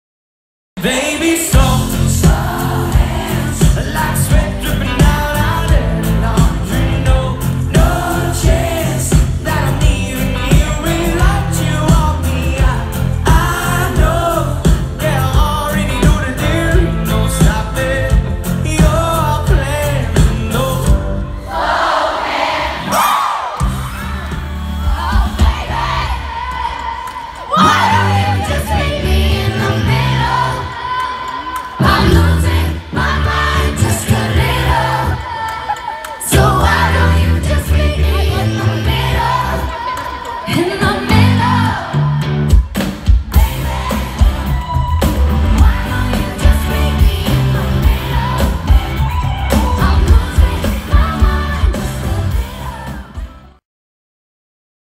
Niall Horan and Maren Morris at Ascend Amphitheater
Niall Horan’s Flicker World Tour came to Nashville July 24, and opening act Maren Morris received a hometown-style welcome from local fans.